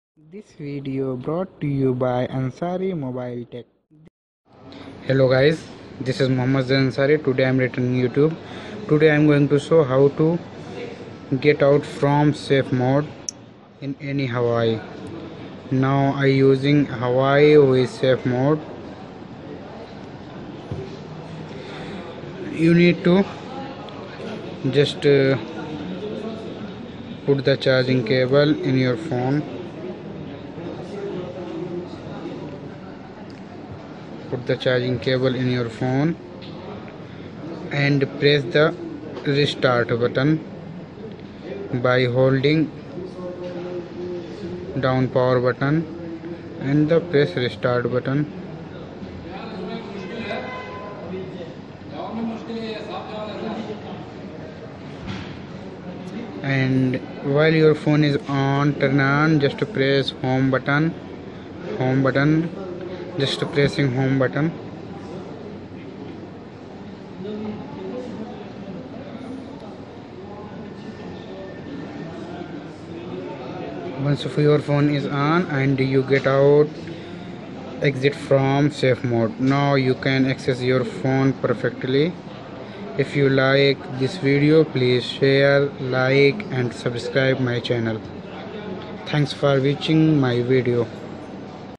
How to exit safe mode in any huawei
in this video i am discribing how to get out from safe mode in any huawei